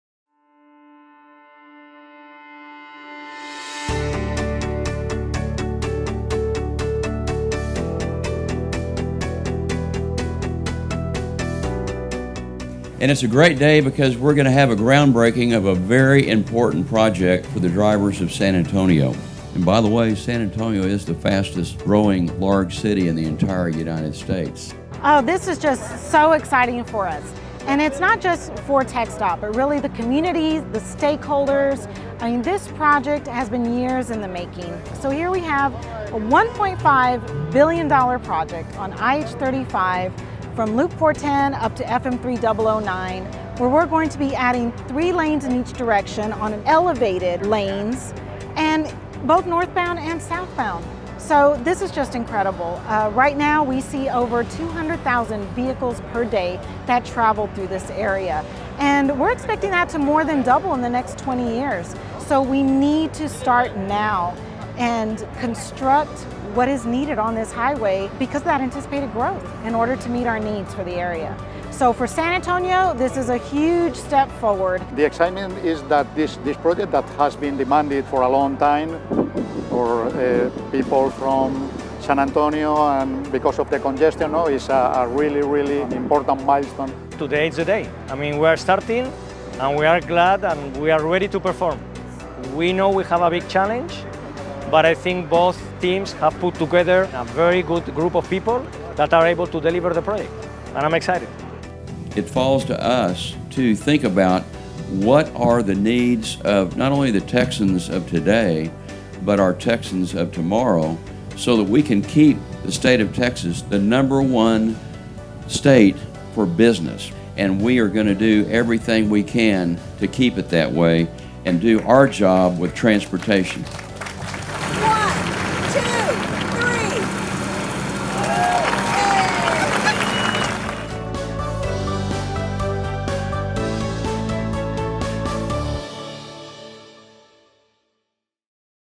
Groundbreaking for IH-35 NEX Central project in Texas | Ferrovial
Texas Department of Transportation, state and local officials commemorated the start of construction on the Interstate 35 Northeast Expansion (NEX) Central project with a groundbreaking ceremony that drew more than 200 guests on May 11th.

The ceremony was led by Texas Transportation Commission Chairman J. Bruce Bugg, Jr. and TxDOT District Engineer Gina Gallegos, P.E., followed by remarks from Texas State Senators Donna Campbell and Jose Menendez, and State Representatives John Kuempel, Barbara Gervin-Hawkins, Steve Allison and Elizabeth “Liz” Campos.